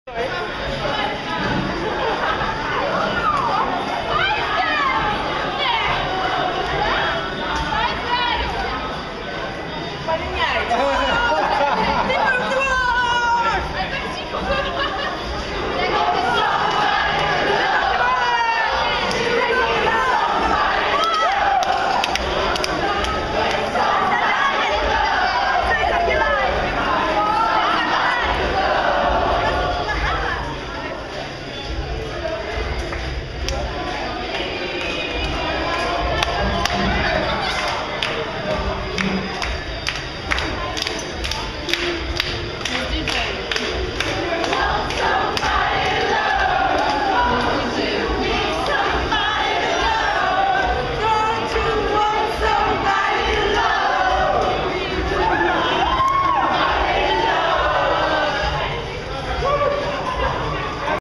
Silent Disco @ Heineken Open'er 2011
Fancy a disco without loudspeakers? But everyone's having lots of fun!